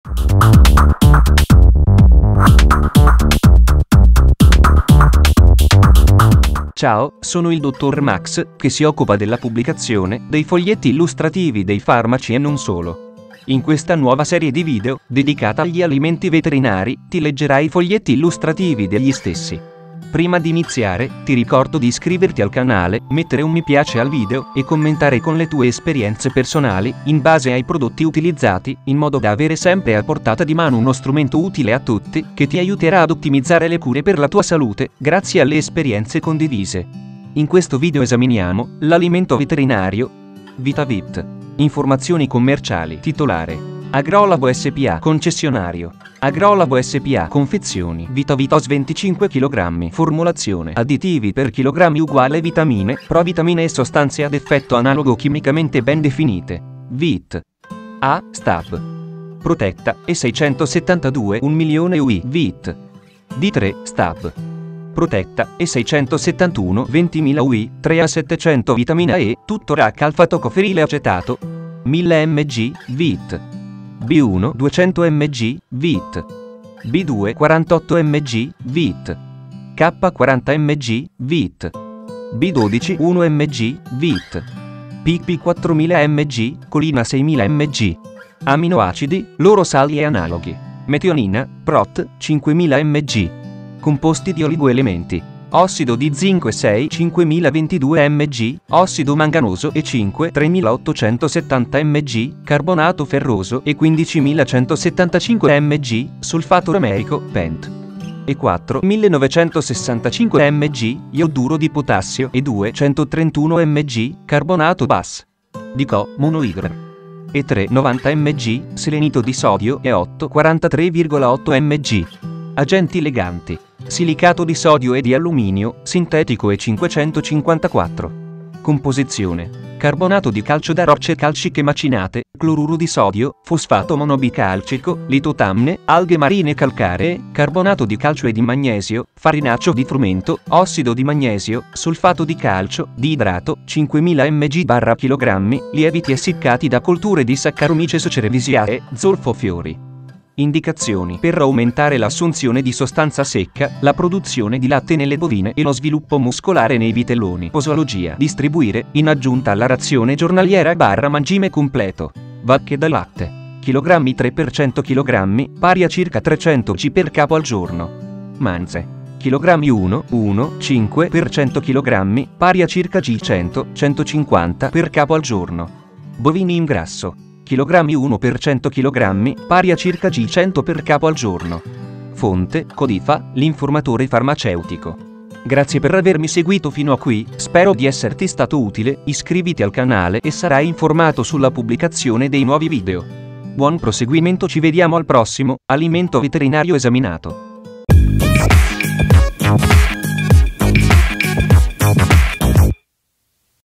🍪 VITAVIT Alimento Veterinario a Cosa Serve ? 🩺 Info Salute Salute
❤️ 🩺 Info Salute: Abbonati a questo canale per accedere ai vantaggi

Abbiamo letto il Foglietto Illustrativo e parlato dell' Alimento Veterinario: VITAVIT

Capitoli ⏱
0:00 Introduzione
0:06 Presentazione
0:40 Alimento Veterinario VITAVIT
1:01 A cosa serve ?

VITAVIT Alimento Veterinario